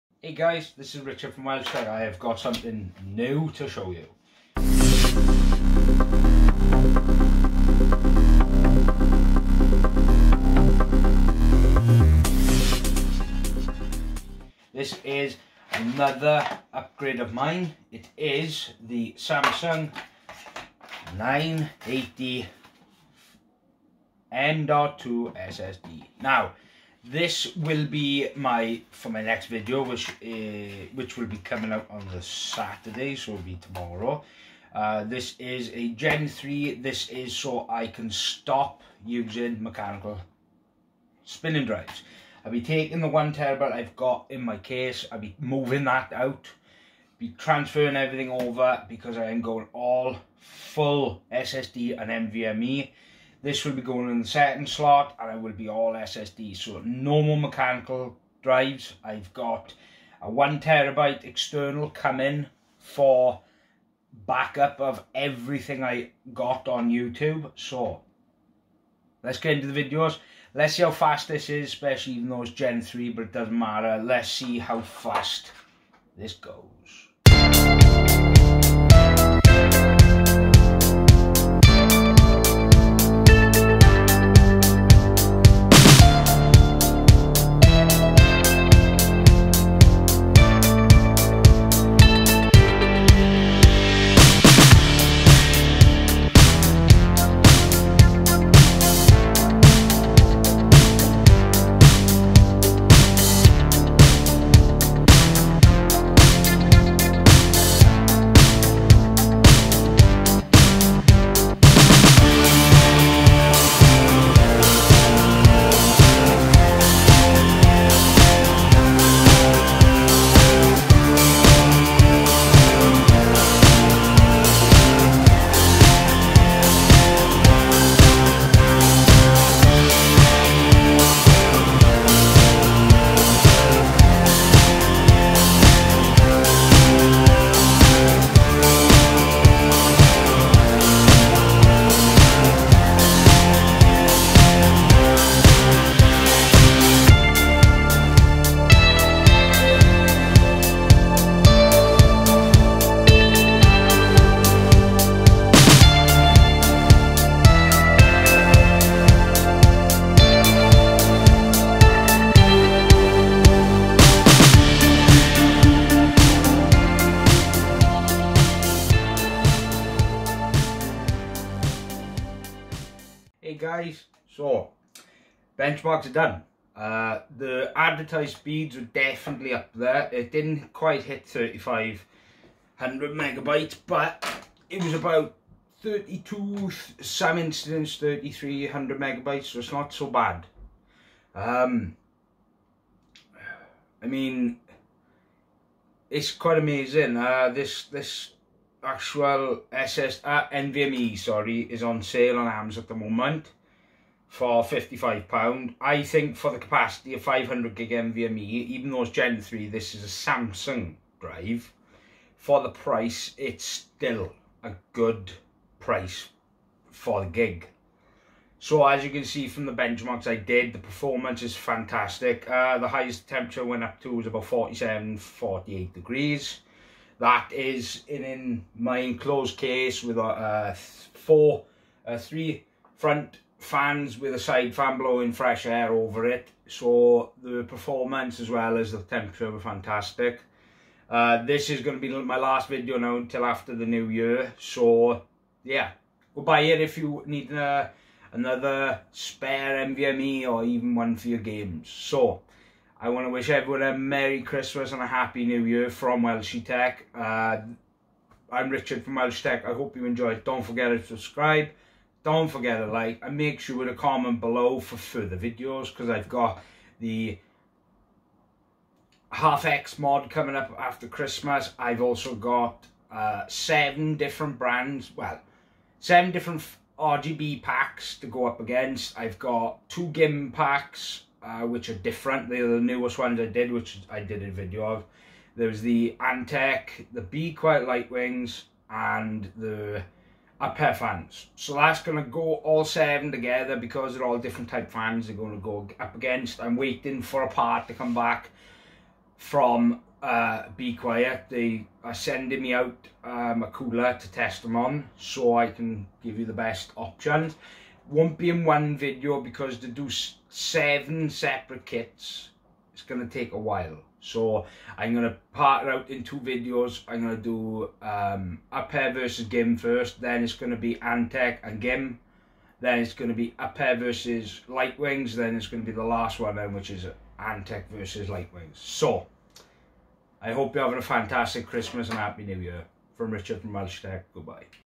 Samsung 980 NVME M.2 SSD Worth it ?
Samsung 980 NVME Worth it ???
Samsung 980 NVME First Look & Benchmarks
I Ran Benchmarks And the Performance is Good For The Price £54
if You Want A Last Christmas gift this is a fantastic value